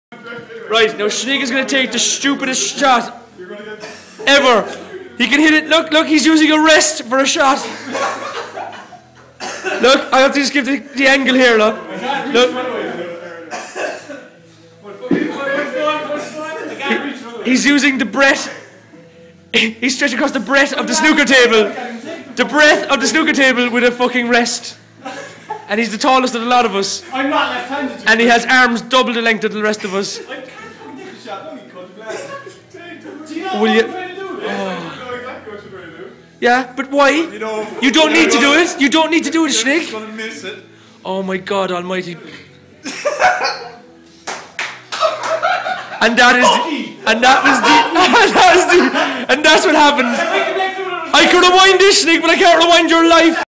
Gangloid- The Rest is History
The worst and most uncomprehendable snooker shot you will ever see- FACT